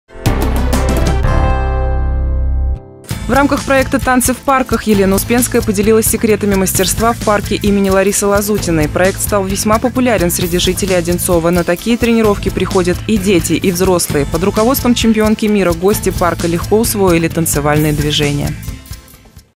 Мастер-класс от чемпионки мира
Елена Успенская провела мастер-класс в Одинцово. В рамках проекта «Танцы в парках» Елена Успенская поделилась секретами мастерства в парке имени Ларисы Лазутиной. Проект стал весьма популярен среди жителей Одинцово. На такие тренировки приходят и дети, и взрослые. Под руководством чемпионки мира, гости парка легко усвоили танцевальные движения.